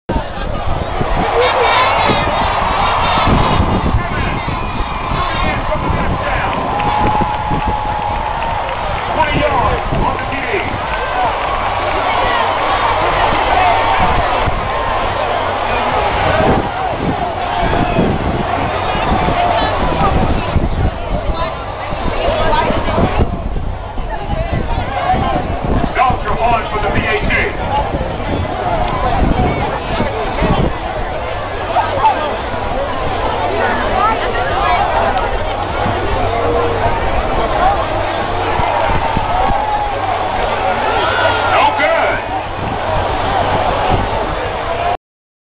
SCSU Tubas Homecoming 2012
South Carolina State tubas walking to the field Homecoming 2012